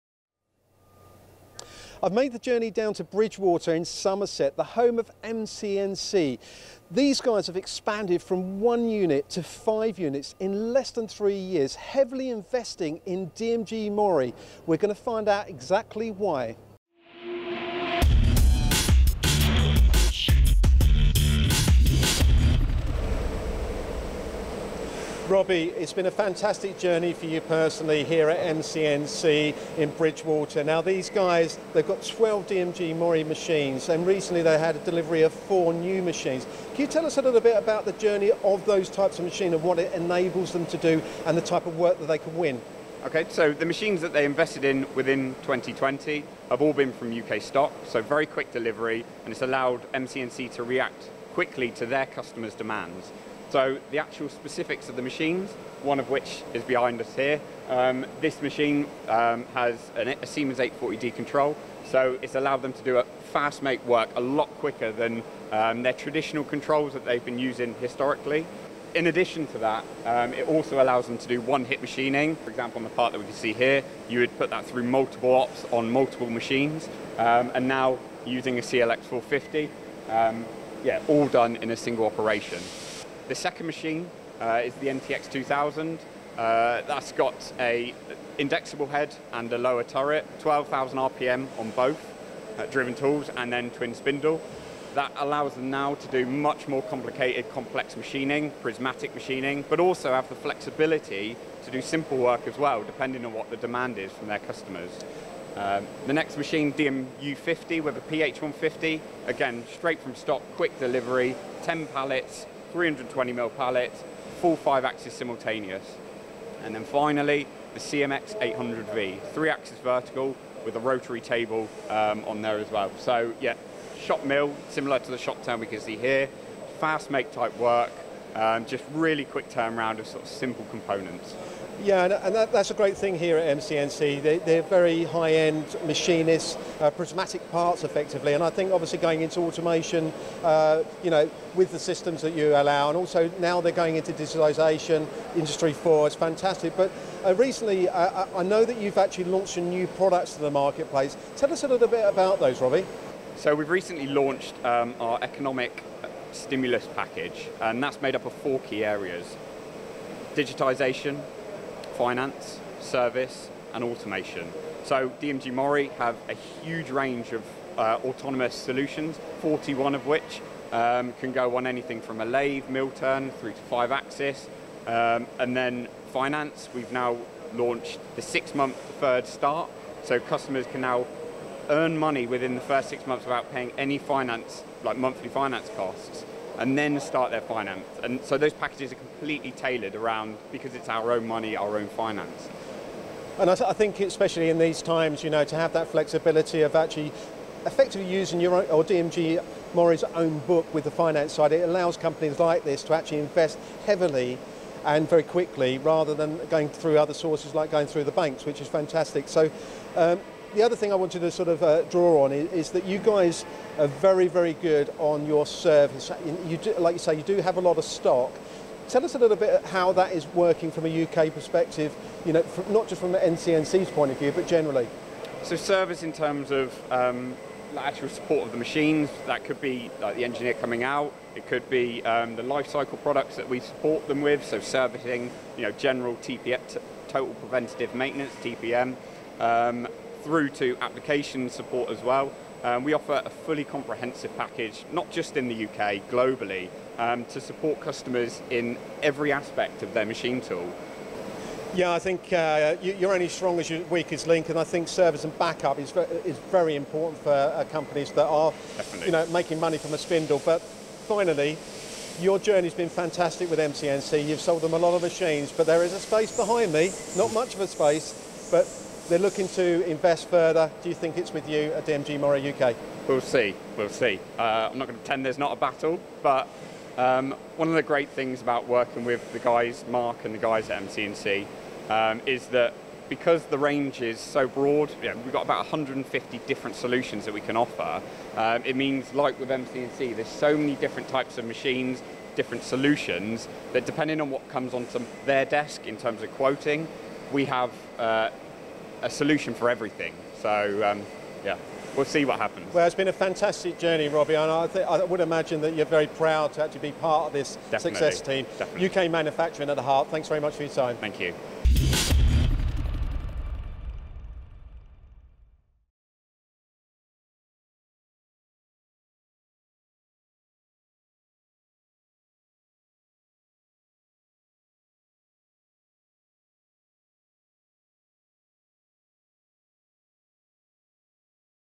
How DMG MORI is supporting UK Manufacturing during these challenging times
Robbie Kelham invited MTDCNC to M-CNC in Somerset to show how their recently launched ‘Economic Stimulus Package’ is already being adopted by UK Manufacturers. Many companies are facing unprecedented economic challenges as a result of the current COVID-19 pandemic. DMG MORI have recognised and reacted to help support these businesses by creating a package specifically focused on automation, digitization, financing and service to allow them to trade out of these difficult times.

M-CNC, one of the fastest most progressive companies within the South-West, have capitalized on this program to strategically position themselves to provide their current and prospective customers with a higher quality, faster and more cost effective solution when outsourcing machined components. Robbie tells the story of their relationship and how they collaborate together to invest in the very best machining solutions for their future growth.